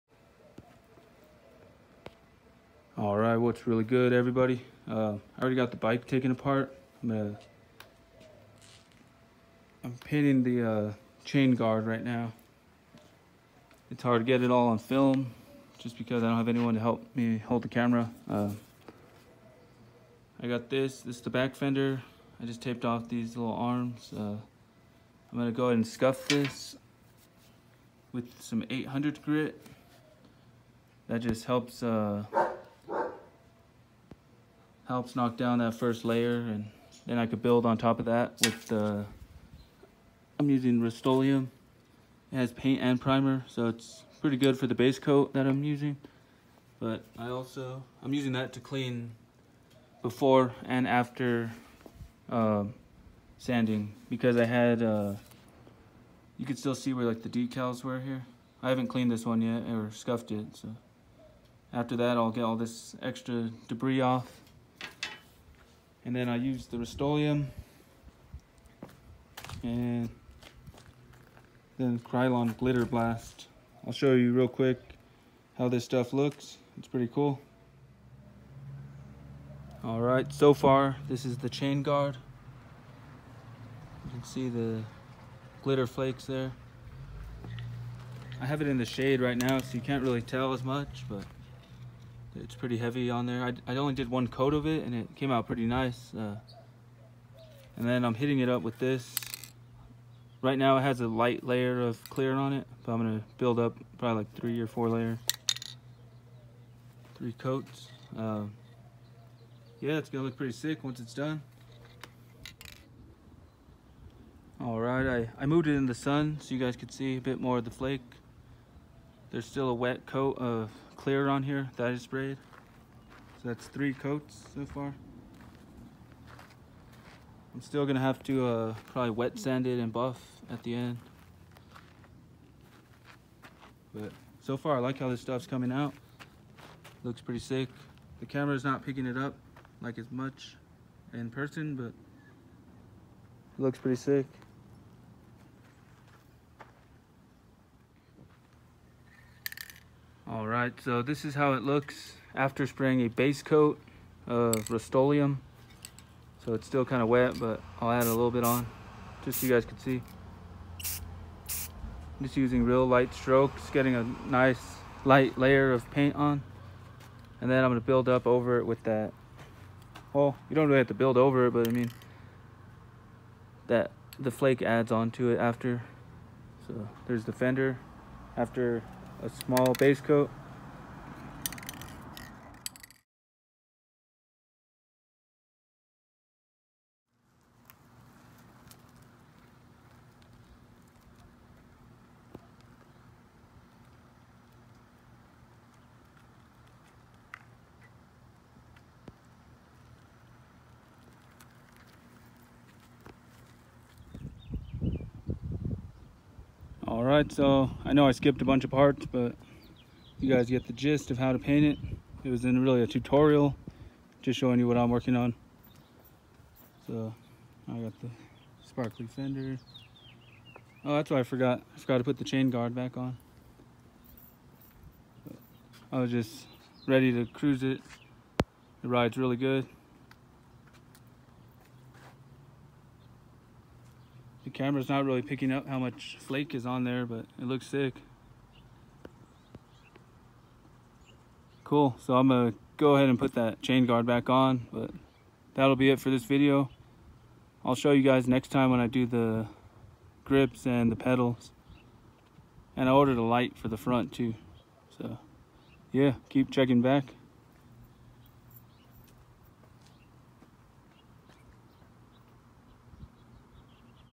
Painting Bicycle Fenders With Krylon Glitter Blast
Quick video of me painting the beach cruiser fenders with krylon glitter blast for a flaked out effect